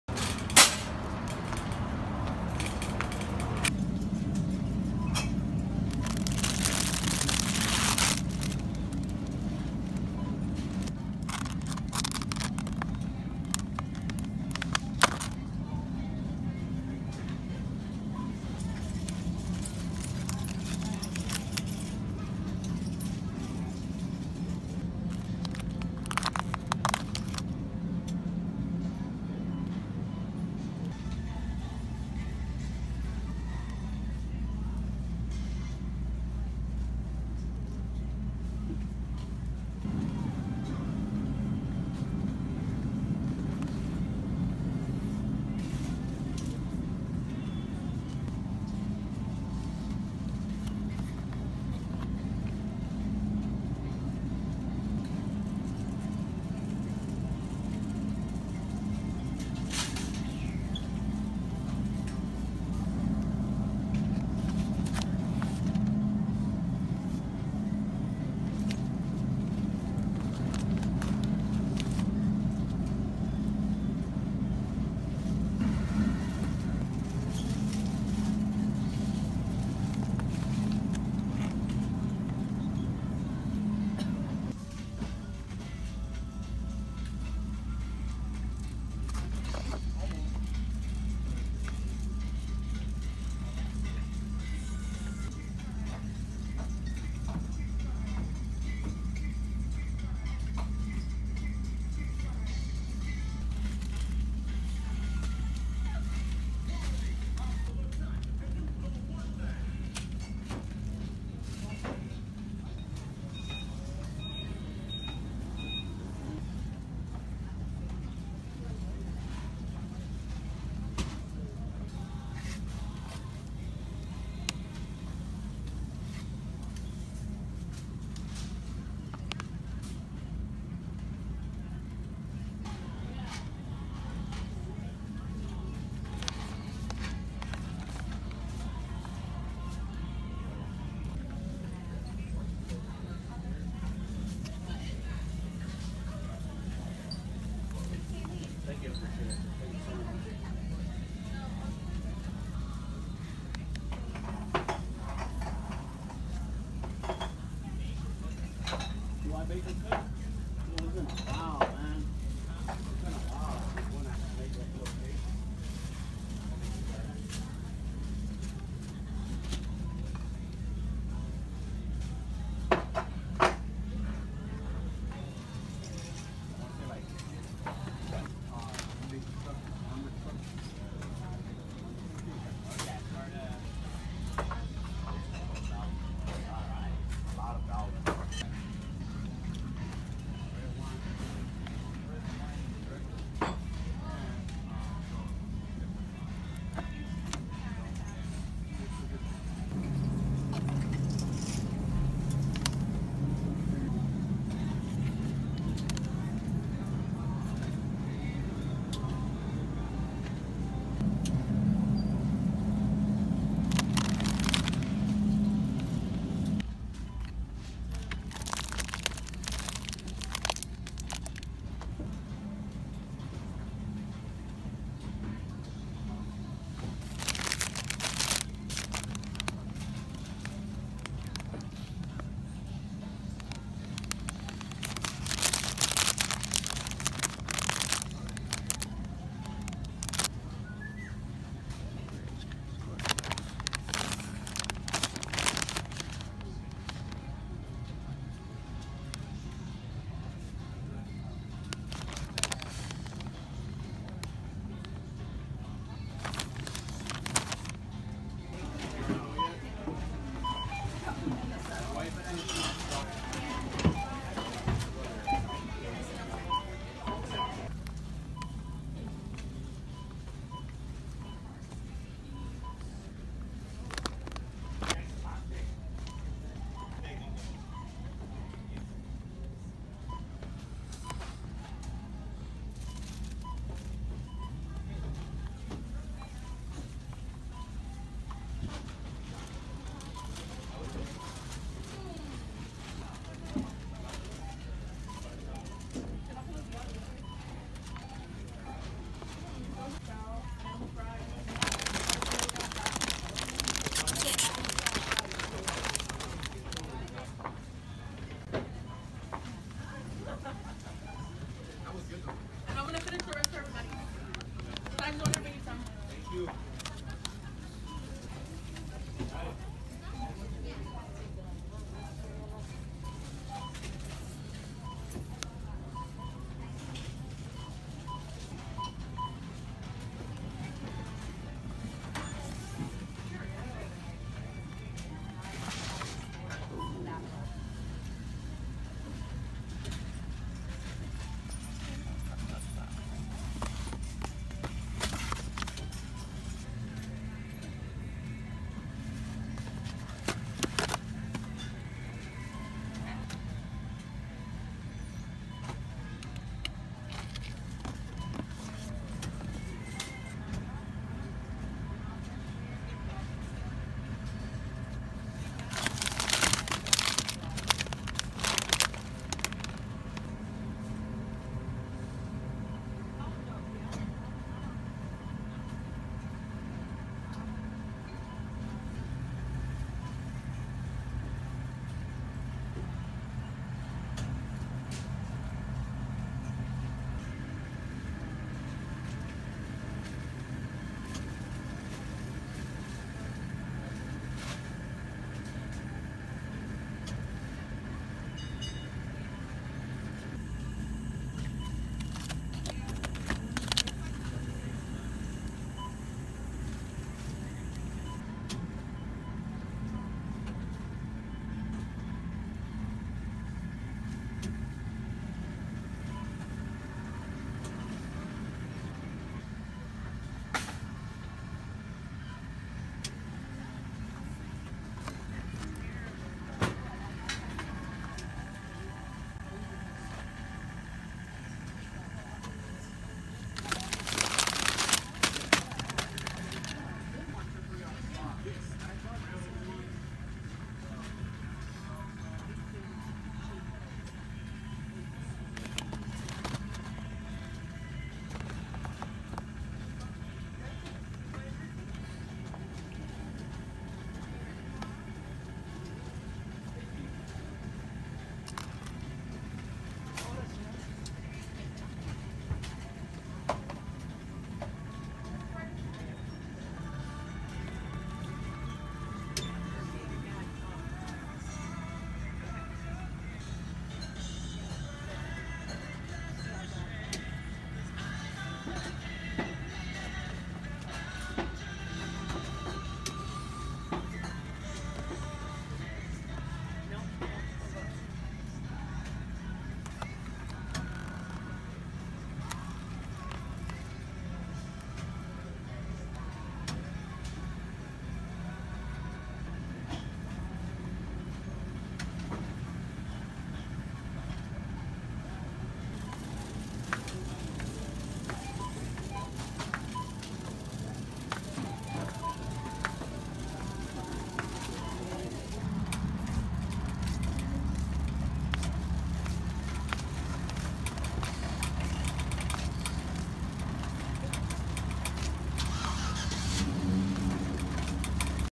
Trader Joe's Thanksgiving 🍁 | Grocery Shopping | Nov 2024
Trader Joe's FALL FAVORITES have arrived with thanksgiving stuffing chips, mini pecan pies, cornbread chips and more.

trader joes, grocery haul, grocery shopping, shopping, shop with me, shopping haul, notalkingvlog, silentvlog